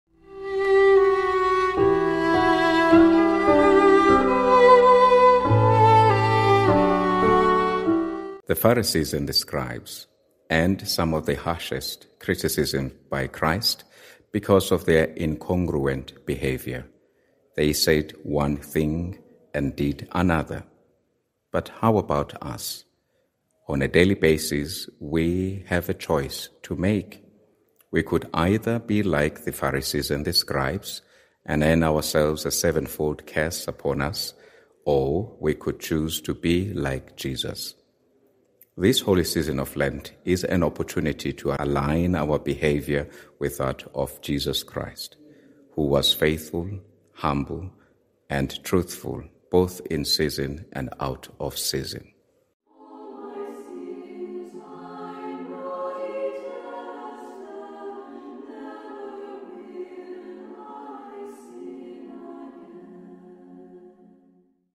Reflection for Tuesday of the Second Week of Lent-Bishop Thulani Mbuyisa CMM of Kokstad Diocese
'Cast away from you all the crimes you have committed, says the LORD,
and make for yourselves a new heart and a new spirit' Ez 18:31 - Lenten Reflection for Tuesday of the Second Week of Lent by Bishop Thulani Victor Mbuyisa CMM of Kokstad Diocese